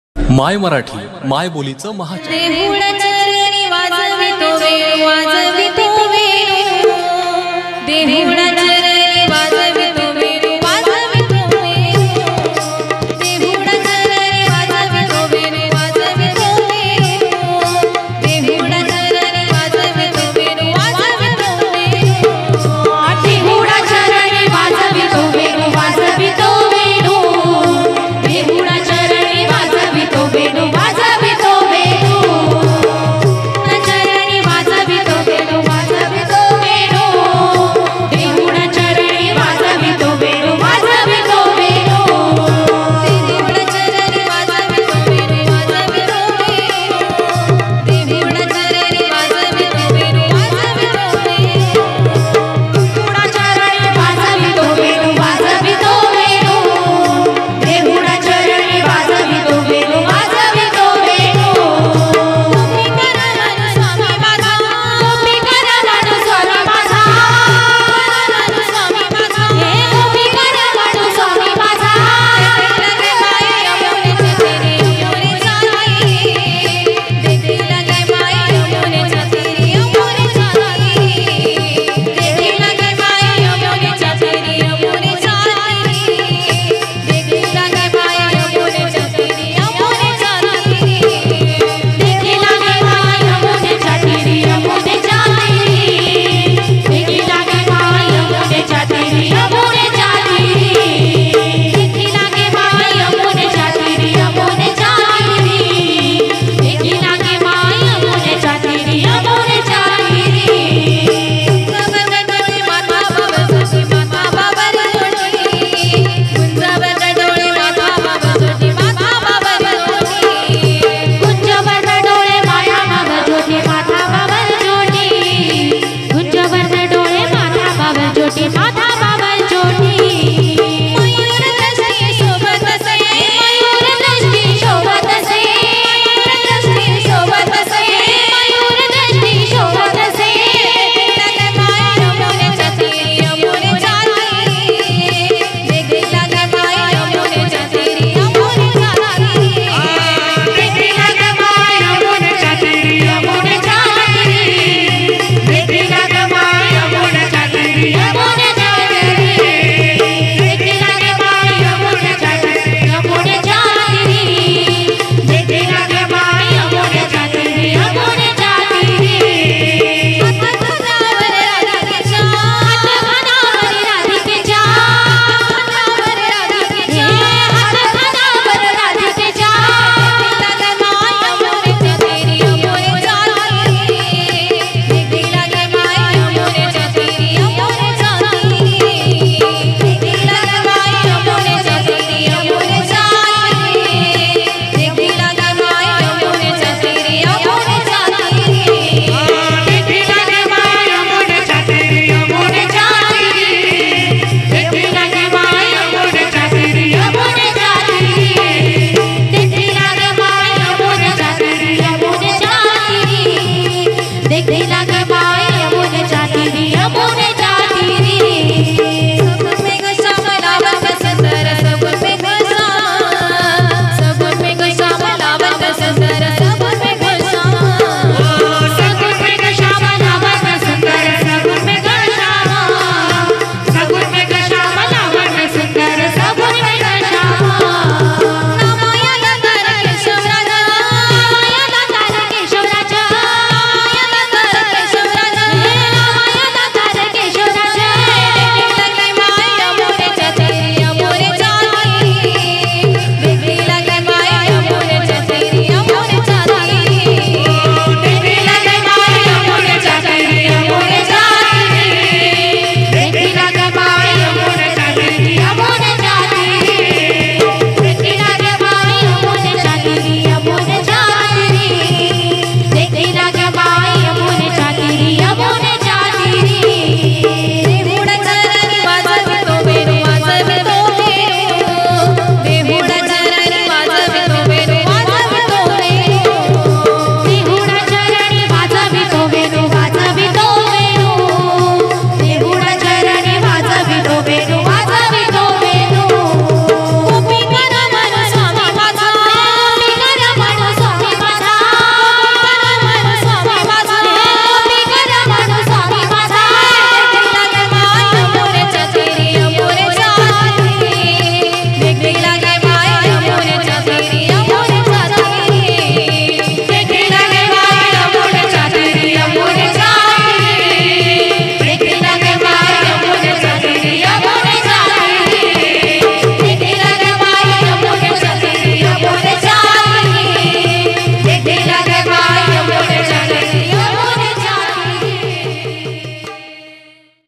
paramparik gavlan,अनुक्षा बदने, anuksha badne,folk song,bhajan,gavlan,dj,भजन,भक्तिगीत,भावगीत,गवळणी,
अनुक्षा बदने ... गवळण गायकीतील उदयोन्मुख चेहरा. अनुक्षाच्या आवाजात लोकगायकीला लागणारा खर्जा असून विश्वनाथ मुंडे सरांच्या मार्गदर्शनाखाली तयार झालेली ती उत्तम गायिका आहे.
अनुक्षाने सादर केलेली ही सदाबहार सुपरहिट गवळण खास आपल्यासाठी.

'माय मराठी '.
मायबोलीचं महाचॅनल. !!

mai Marathi...
the biggest channel of the mothertoung.
-rajesaheb kadam